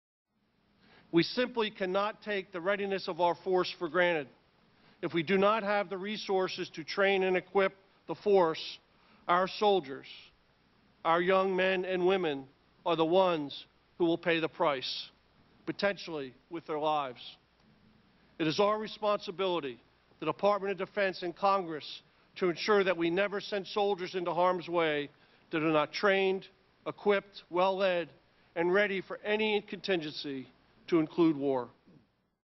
General Ray Odierno's Senate Armed Services Testimony: Our Responsibility
Gen. Raymond T. Odierno testified in front of the Senate Armed Services Committee on February 12, 2013. In this video clip, Odierno discusses the responsibility of Congress and the Department of Defense.